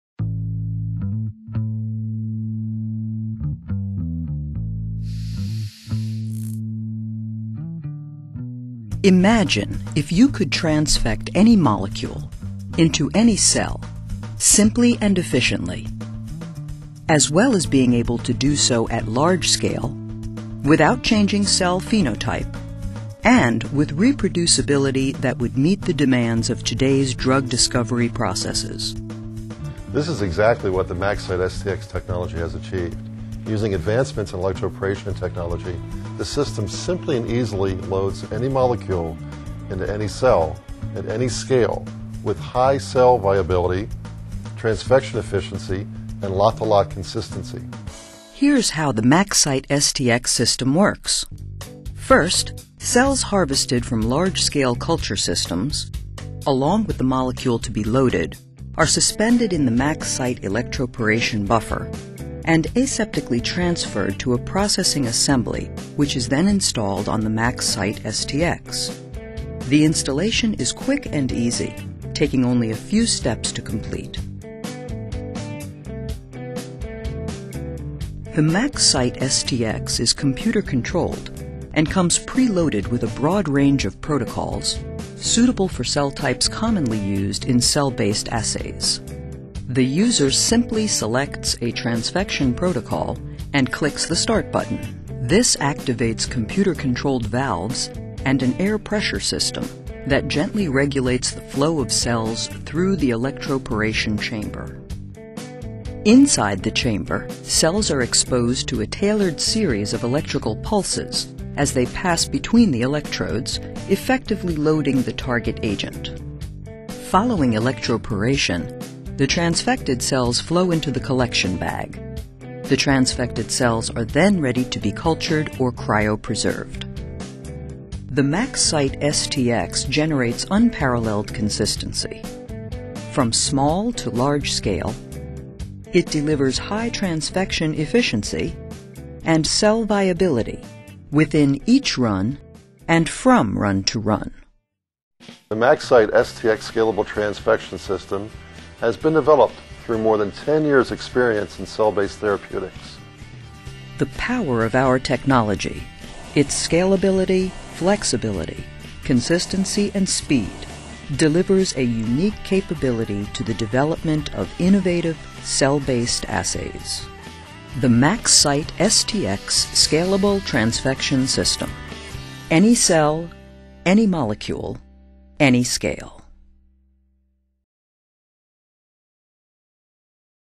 MaxCyte_STX_Introduction.wmv
The MaxCyte® STX™ Scalable Transfection System enables rapid development and consistent production of transfected cells for use in High Throughput Screening (HTS), High Content Screening (HCS), and protein production.  It eliminates the reliance on stable cell lines and gives users the flexibility to (co)transfect cell lines, primary cells, and stem cells with DNA, RNA and proteins. Transfection efficiencies are routinely greater than 85% and cell viability greater than 90%. The MaxCyte STX can perform small-scale transfections (5E5 cells in seconds) for basic research and assay development or perform large-scale transfections (up to 1E10 cells in less than 30 minutes) for use in protein production or for full-scale, screening and profiling.   It can help you develop assays faster, and in more biologically relevant cell types, increasing the productivity of your laboratory and improving the quantity and quality of your drug candidates.